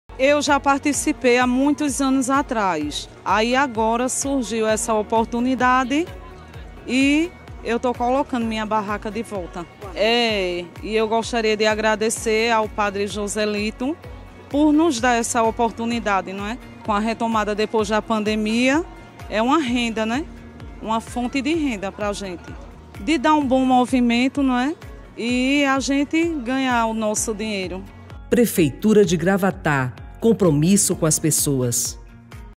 Prefeitura de Gravatá garante serviços essenciais para segurança e conforto na Festa de Reis
Prefeitura de Gravatá garante serviços essenciais para segurança e conforto do grande público na Festa de Reis.

A tradicional festividade atrai multidão ao Pátio de Eventos e por isso atendimento de saúde, segurança e estrutura são tão importantes quanto os shows no palco.